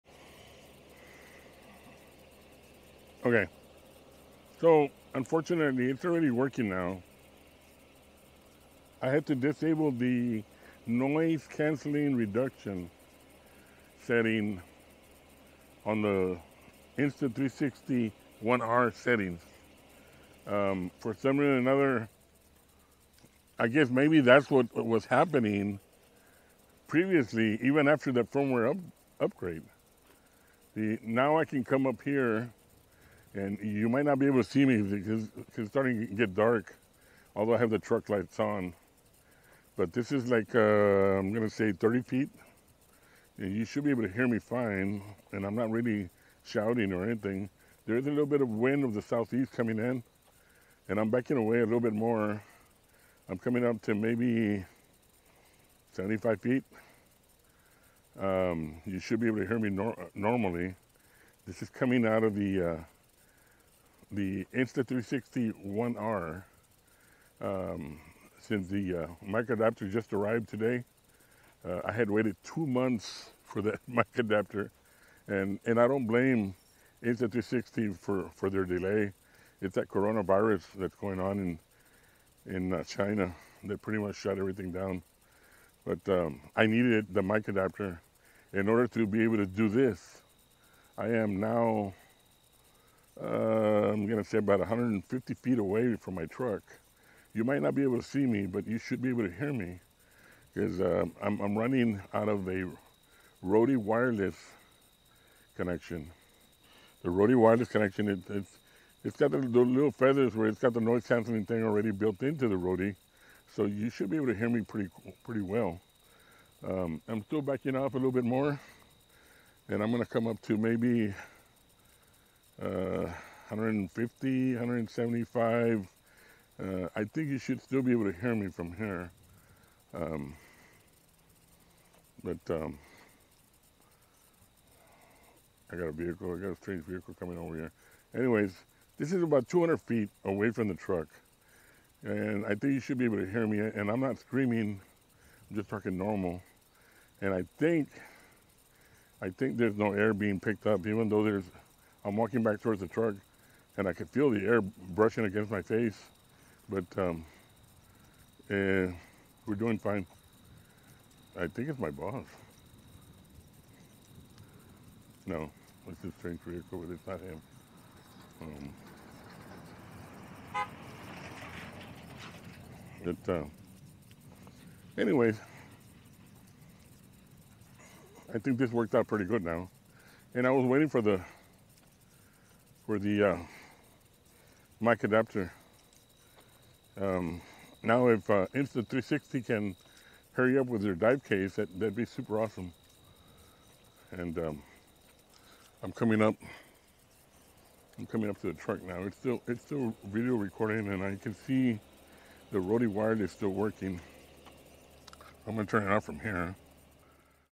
Insta360 One R (mic Adapter)
Although the video got a little blurry because I was using the recapture function , the idea of this video was to test the mic adapter connected to the Rode wireless transmitter.